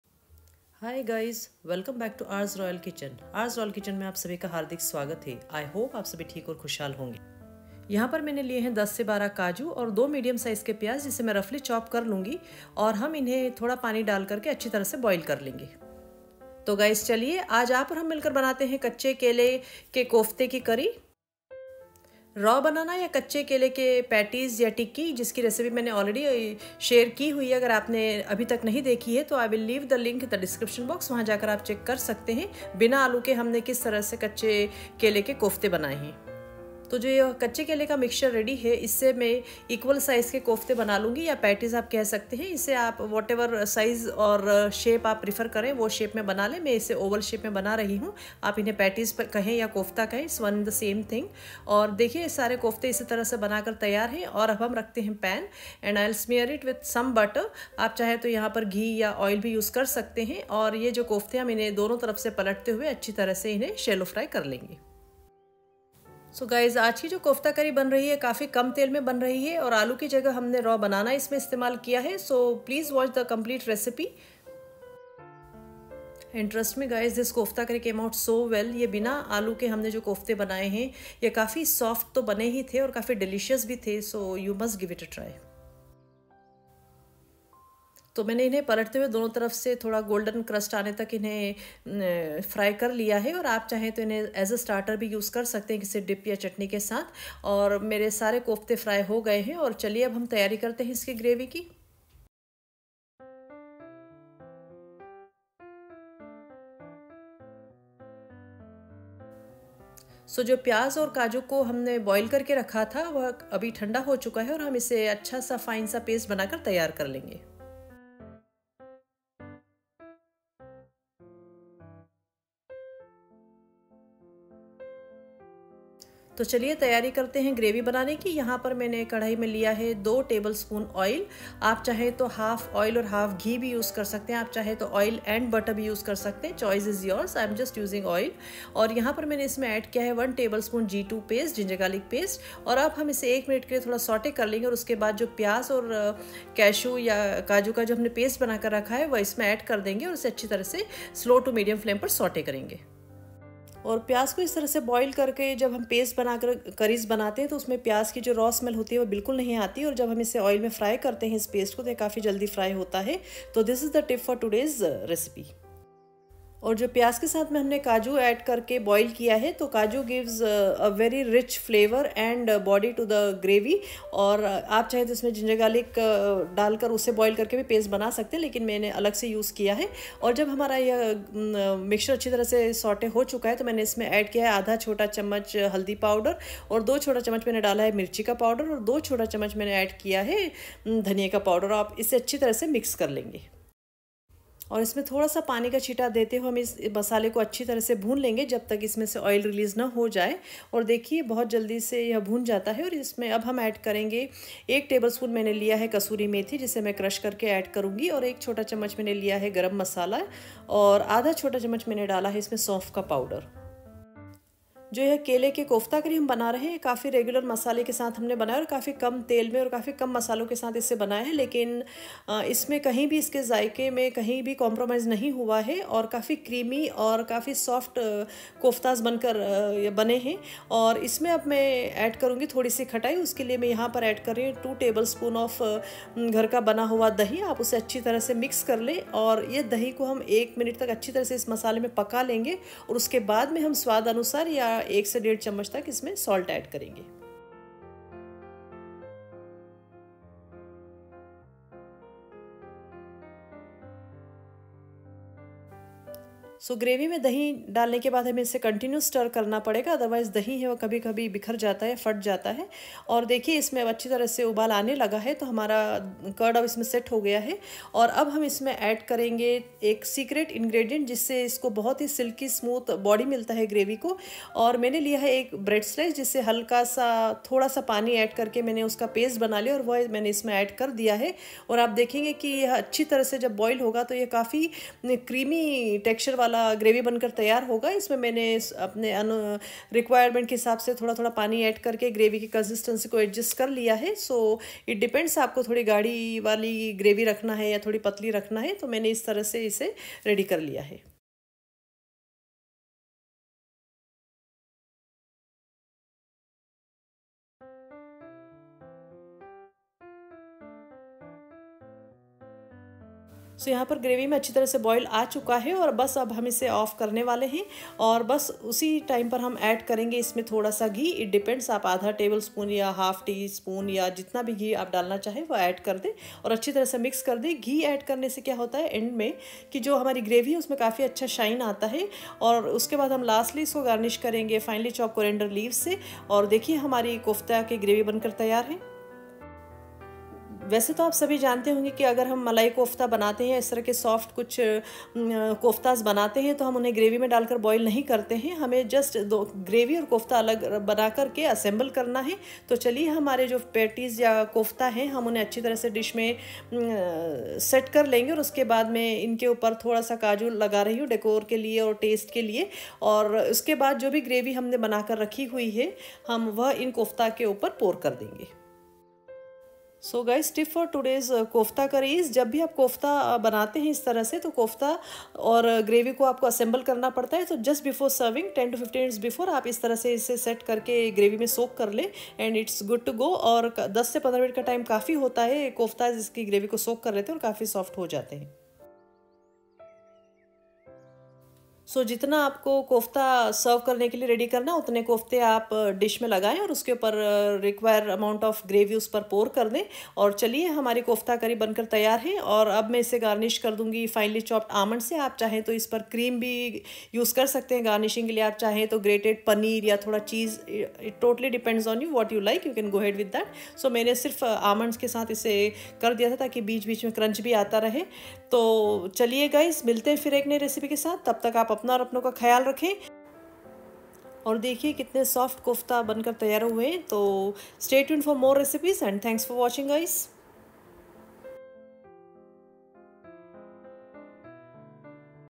Bina aloo..mooh me ghulne wale kofta curry recipe..less oil cooking recipe
Aloo ke bina…mooh me ghulne wale kofta curry recipe..less oil recipe ​⁠@RRKitchen9